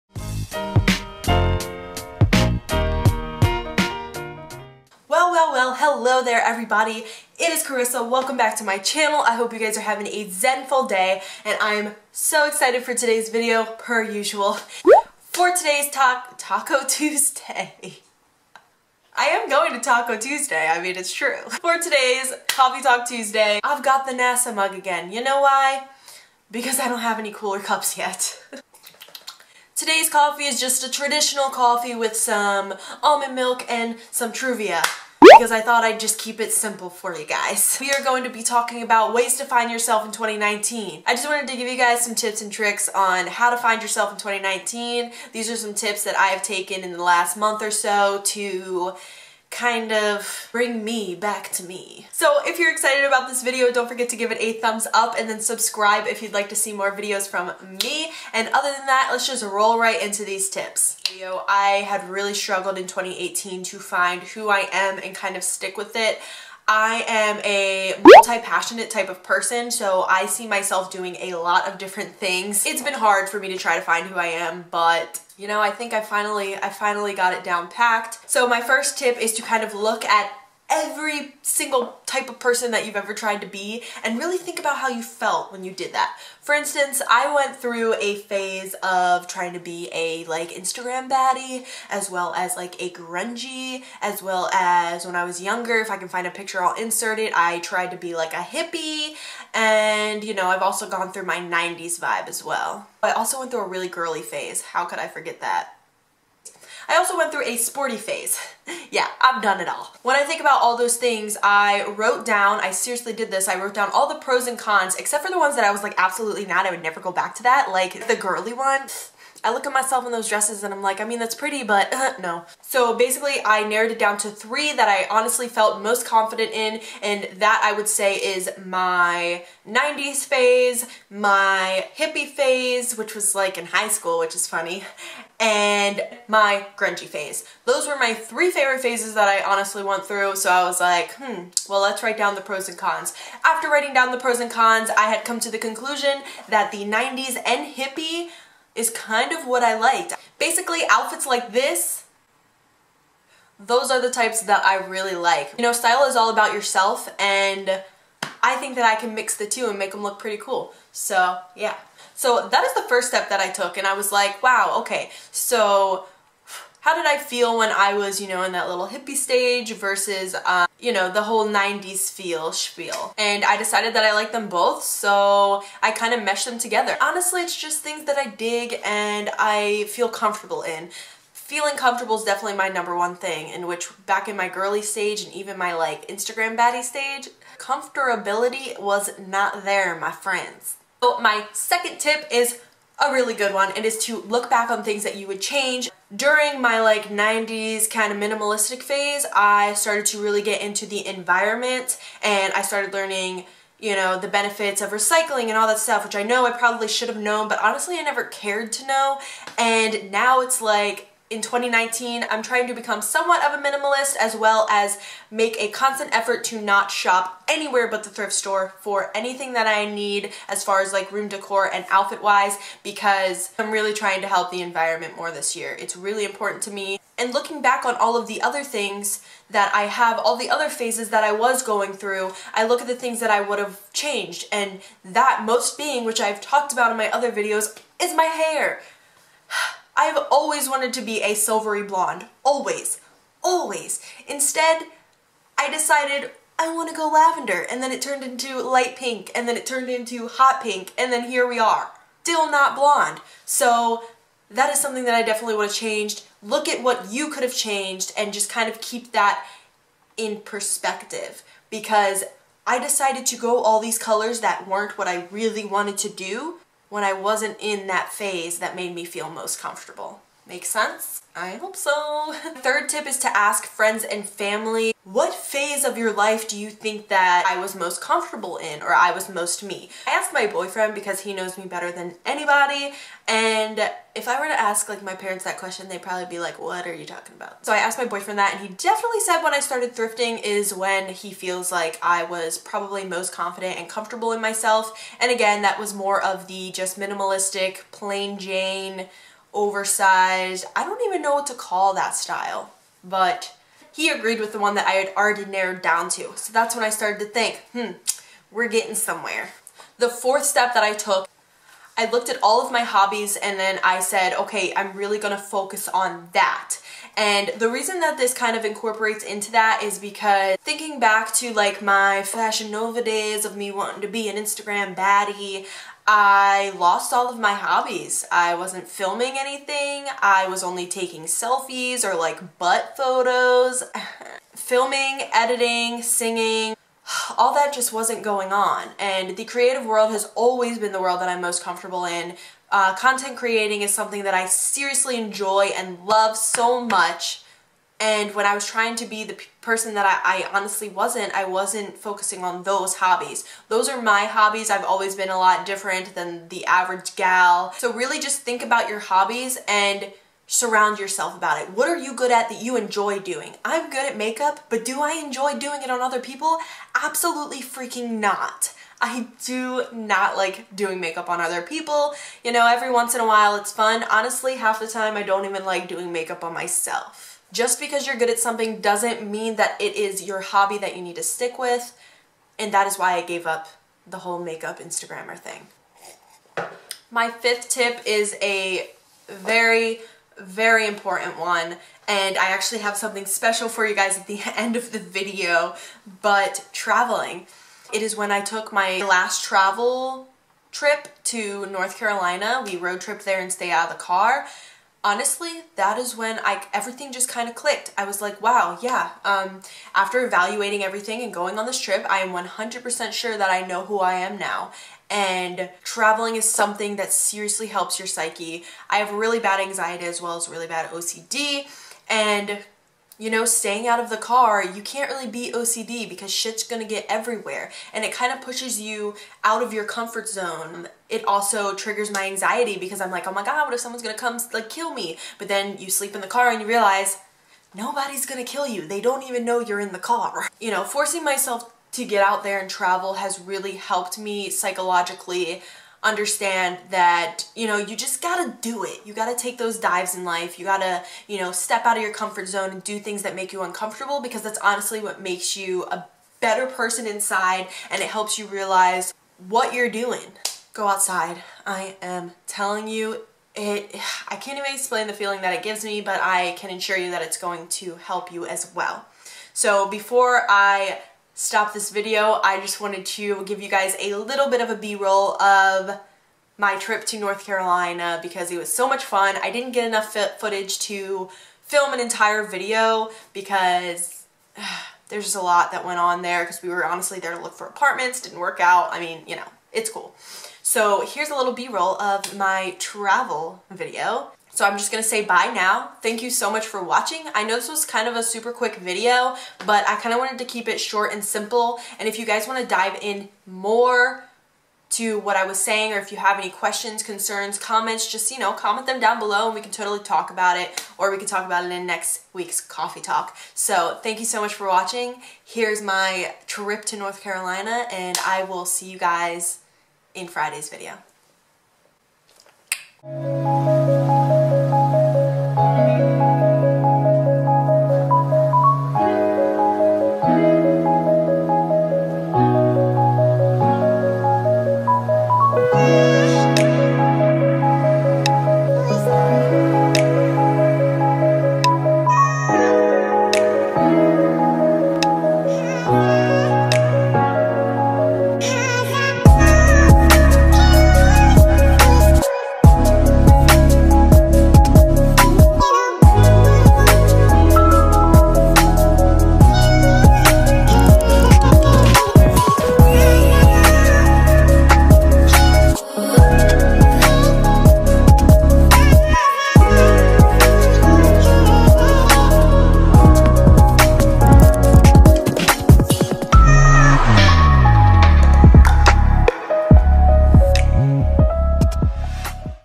HOW TO FIND YOURSELF IN 2019
c  h  a  t   w  i  t  h   m  e  ☽

✰  Upload schedule // Tuesdays & Sundays

2018 was a hard year for me! Being 21, its hard to really know who you are or who you want to be. Last year alone I went through so many different phases trying to dress and act a certain way. Here are some of the ways that I learned how to really find myself in 2019 so far!!!
 ***BTW SORRY IM A DAY LATE IN UPLOADING. wont happen again !